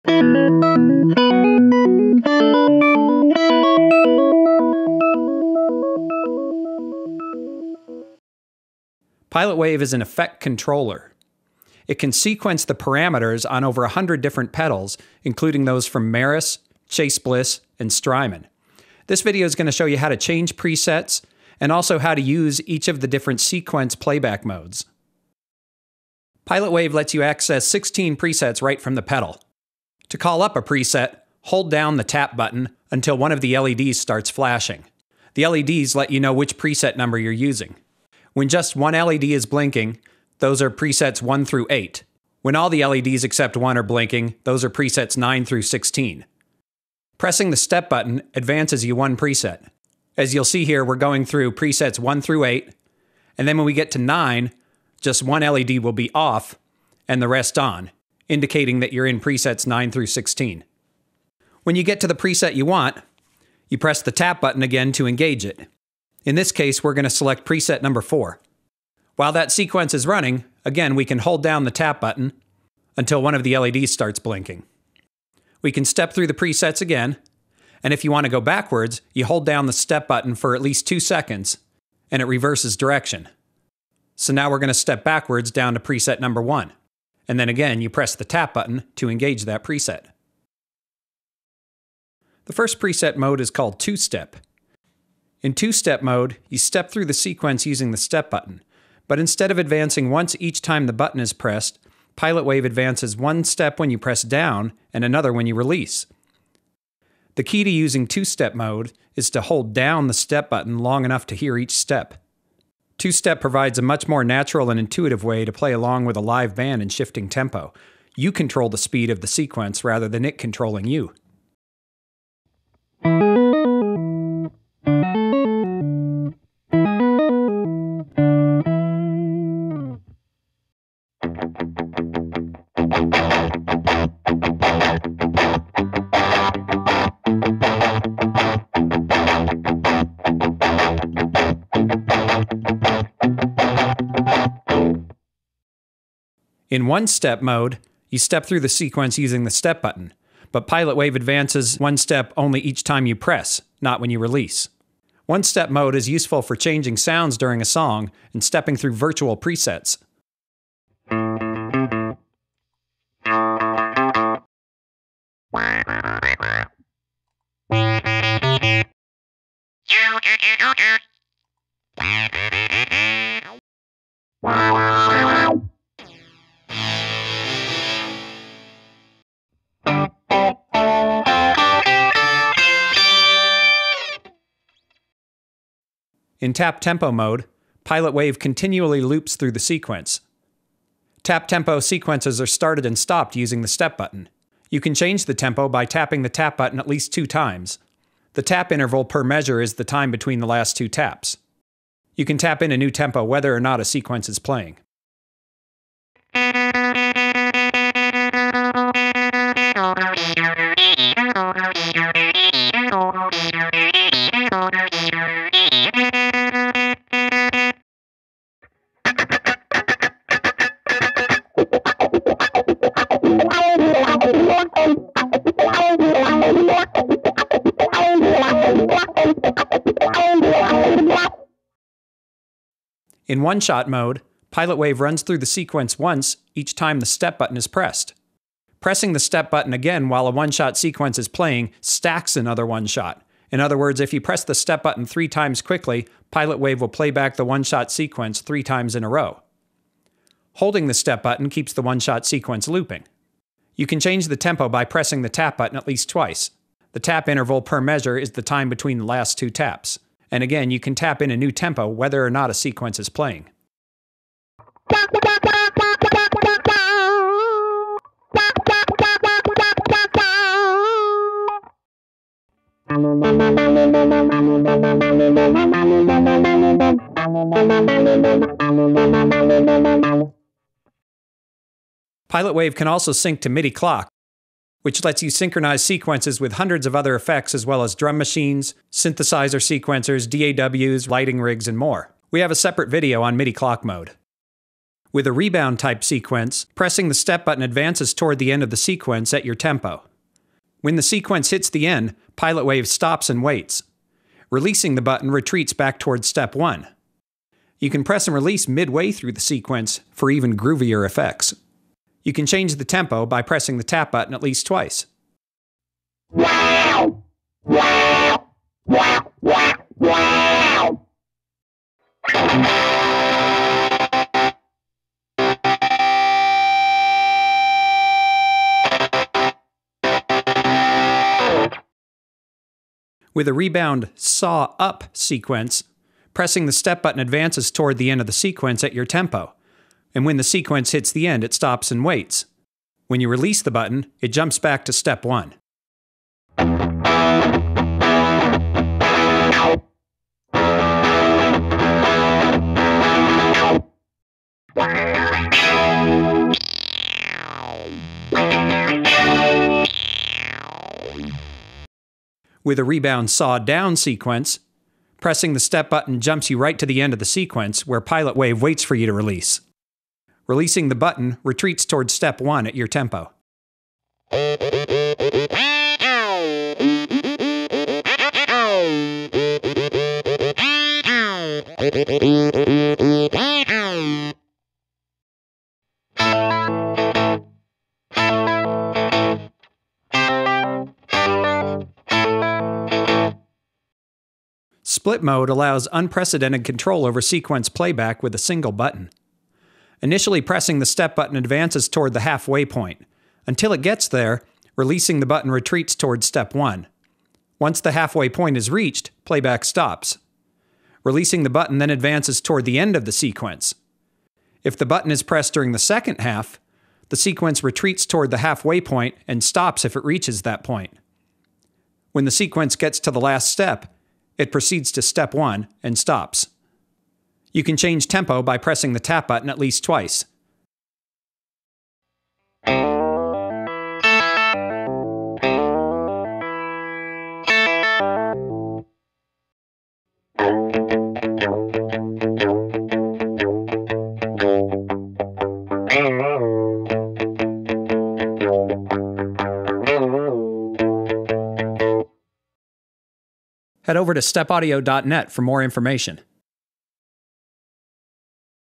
Pilot Wave™ | Selecting Presets and Playback Mode Demo
Pilot Wave™ by Step Audio multiplies the sonic potential of every MIDI-enabled effect. Pilot Wave sequences effects parameters, unleashing hidden pedal potential.

This powerful new controller and patented technology turns any MIDI-enabled pedal into a dynamic, rhythmic musical instrument.

This video demonstrates how to change Presets on the pedal and how to use the nine unique Playback Modes.

Step Audio | Patented Pedals